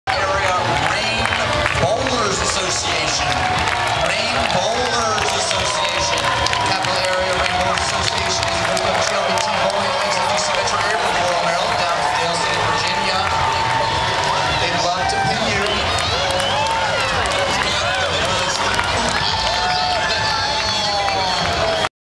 The Capital Area Rainbowlers Association
The Capital Area Rainbowlers Association (CARA) is a member supported organization composed of gay and lesbian bowling leagues in the Metropolitan Washington, DC area.

Video By
Ryan Janek Wolowski